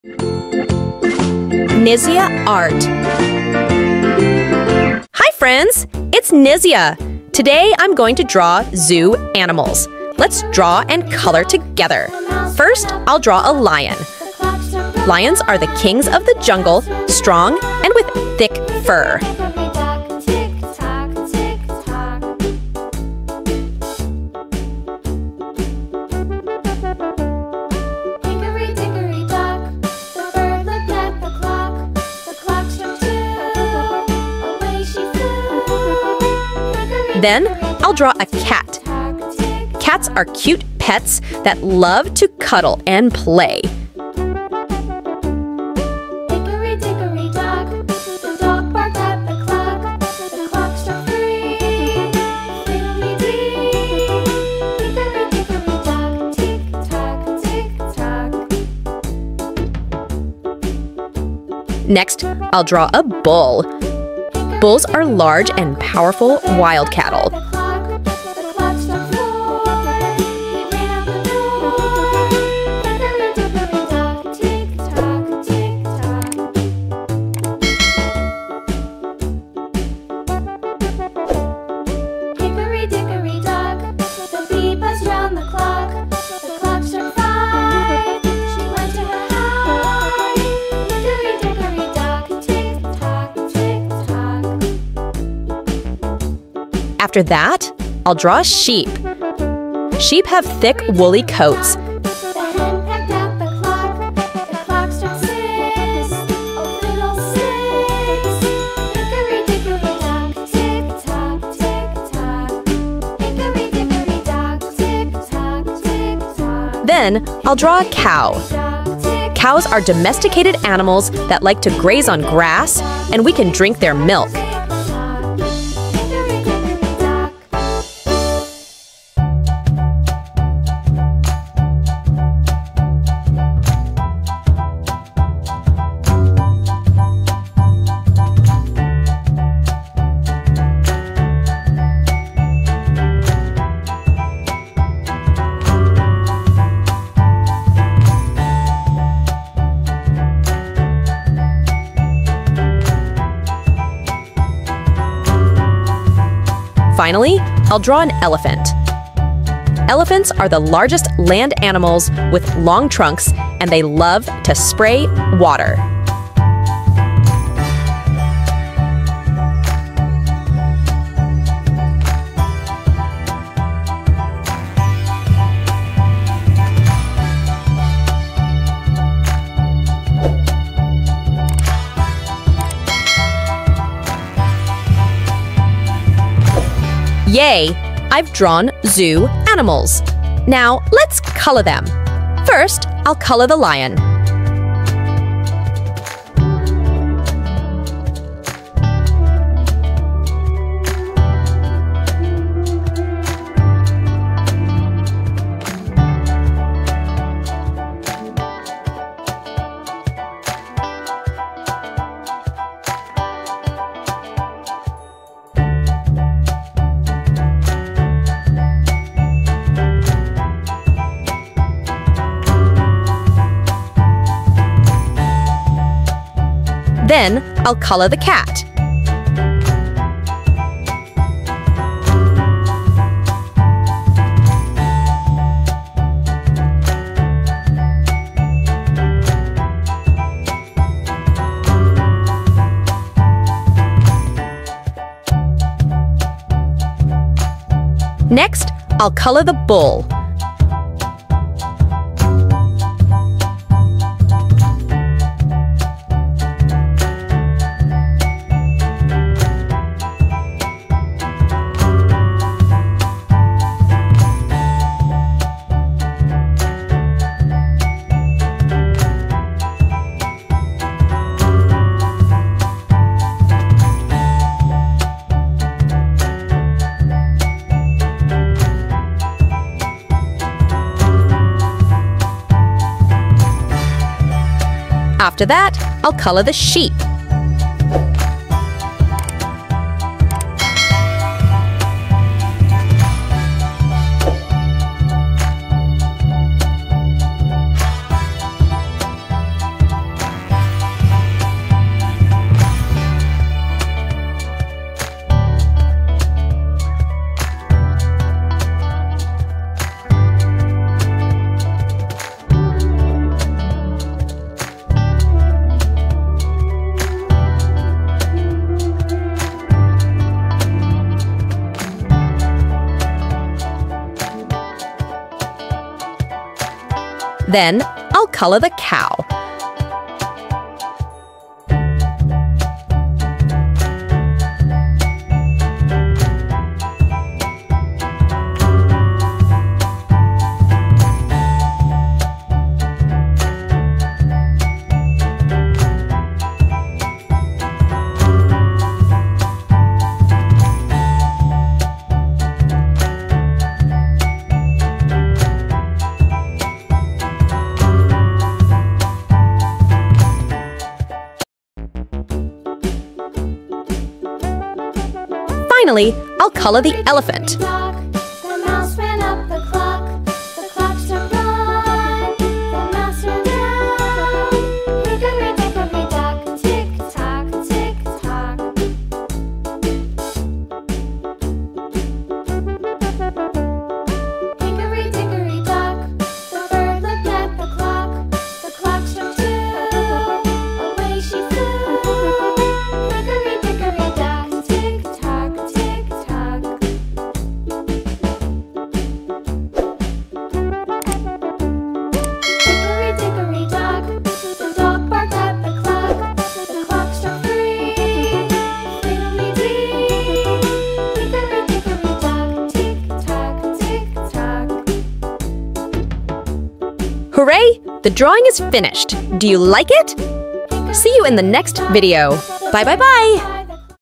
How to draw Zoo Animals - Lion Cat Bull Sheep Cow Elephant for Kids & Toddlers
Learn how to draw and paint Zoo Animals - Lion Cat Bull Sheep Cow Elephant for Kids & Toddlers